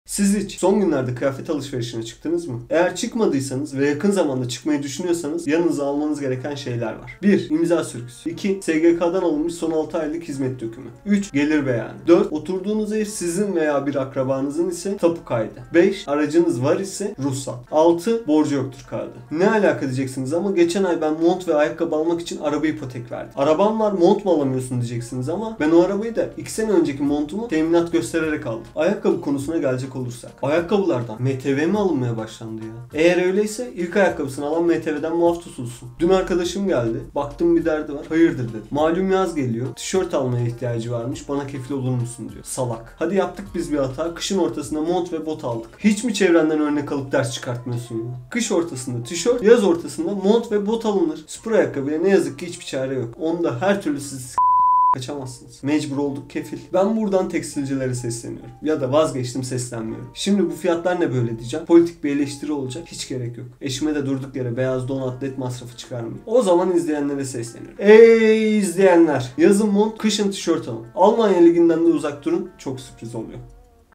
ÇIPLAK ADAM - Kredi Çekerek Kıyafet Almak..
İçinde bulunduğumuz dönemde artan kıyafet fiyatları herkesin malumu. Alışveriş kredileri ile birlikte mont, bot ve hatta tişört alırken bile kredi çeker hale geldik. İşte bugün de bunu tiye alıyoruz!

İyi seyirler.